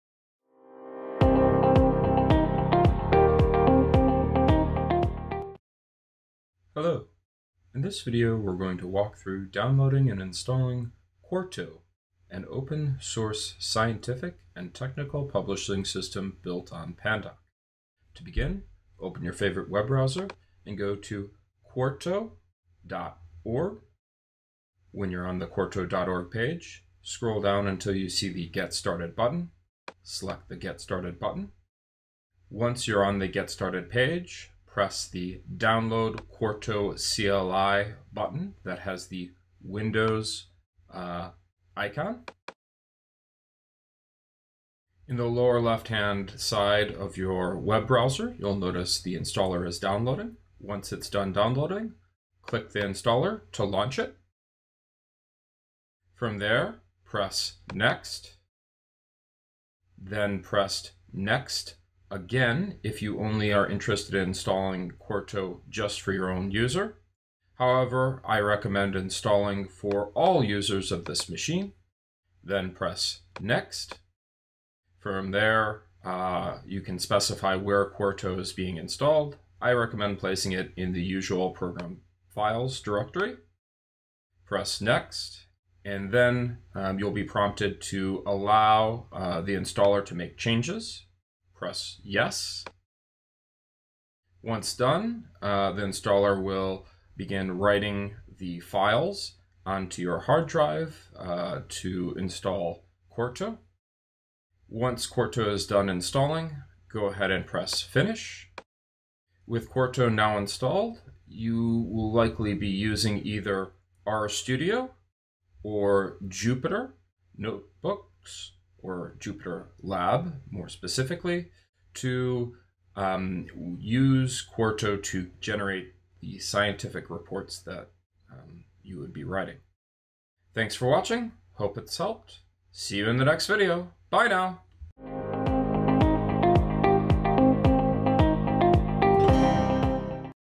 Downloading and Installing Quarto on Windows
Summary

Open Web Browser

Click on the "Get Started" button

Select "Download Quarto CLI"

Once done downloading, open the installer

Follow the installer prompts

Begin using Quarto in any supported editor.